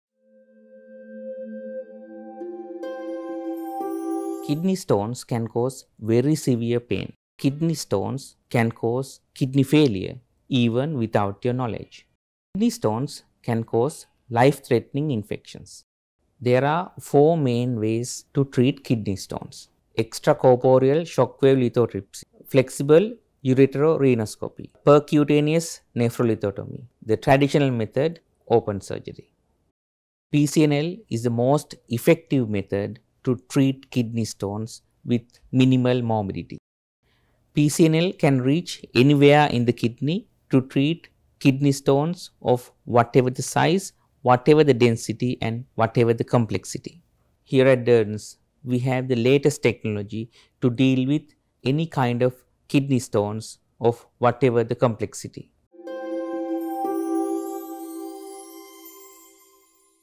DURDANS HOSPITAL - Dr Kalana Interview (English)
60 Second Social Media Cutdown ENGLISH VERSION
Dr. Kalana Parana Palliya Guruge - Urologist
Urology Department for Treatment of Kidney Stones & other Kidney related diseases.

Durdans Hospital PLC
3 Alfred Place
Colombo 00300

Video Production by:
annoyboy Pictures
36/1 Sujatha Mw
Kalubowila
Sri lanka